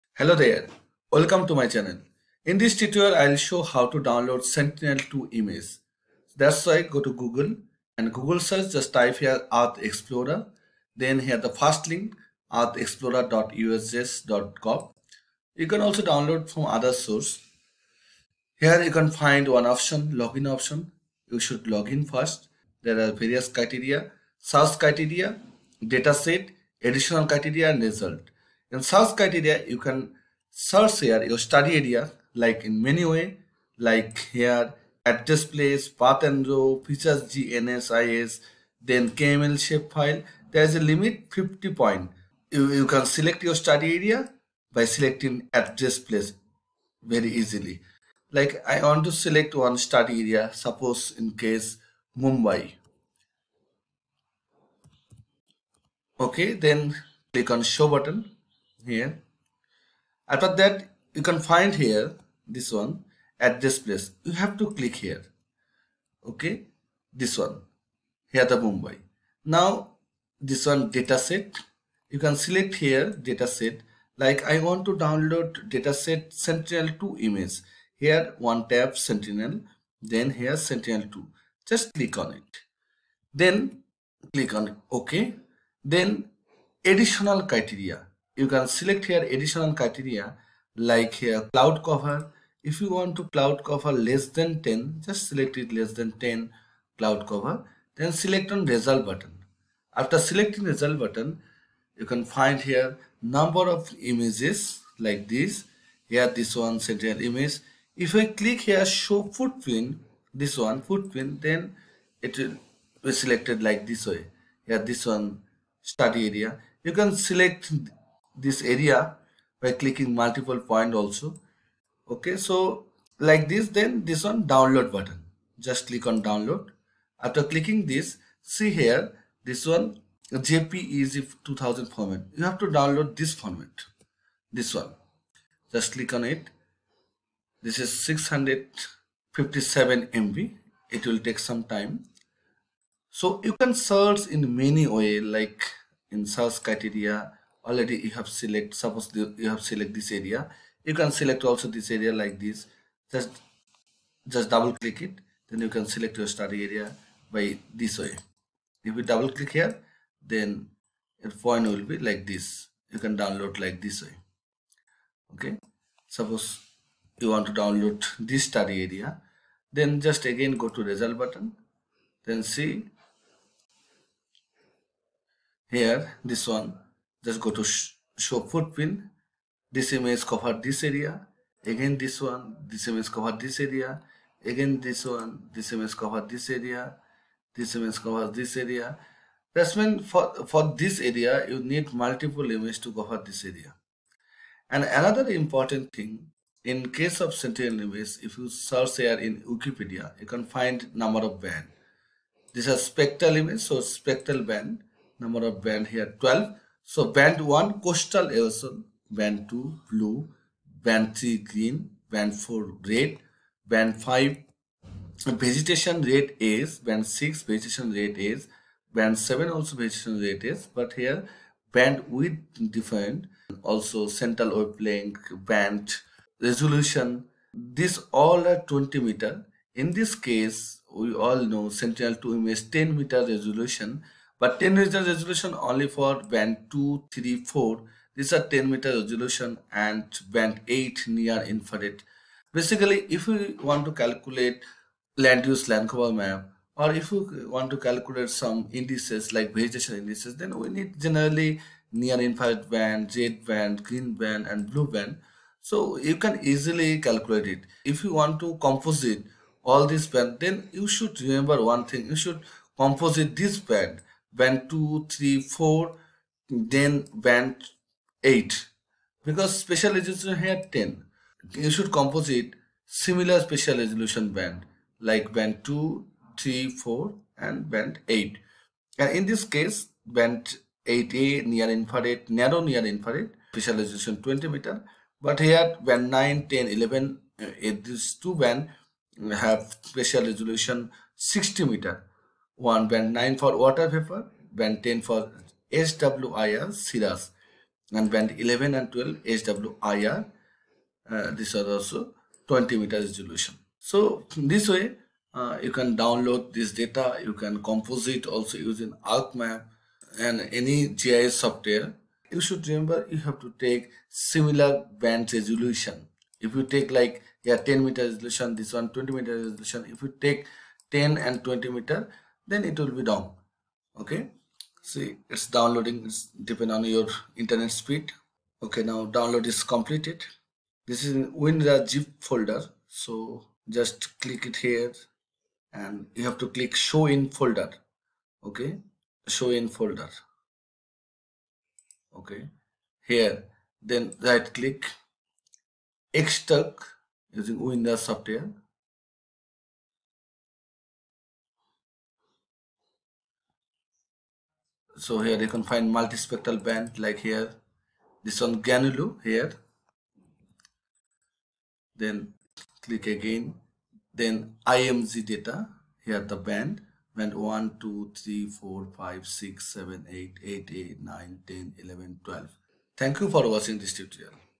How to download Sentinel 2 images ?
In this tutorial, I have shown how to download 10 m Sentinel images and explained band information.